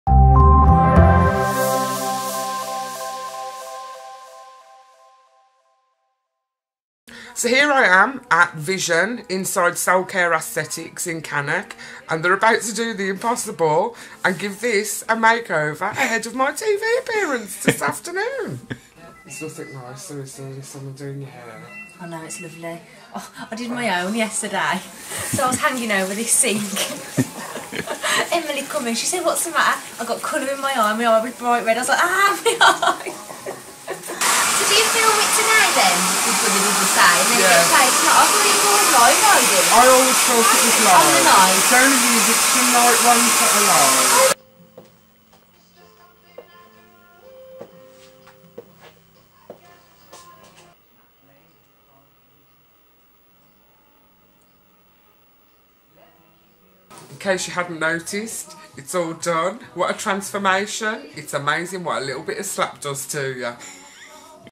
Benefits Street's White Dee made up to be on Big Brother's Bit on the Side
From Benefits Street to High Green Court - reality TV star Dee Kelly was in Cannock to have her hair and make up done before a TV appearance.

The former Celebrity Big Brother star, best known by her nickname White Dee, visited Soul Care Aesthetics to get ready for a guest spot on the spin-off programme Big Brother's Bit on the Side.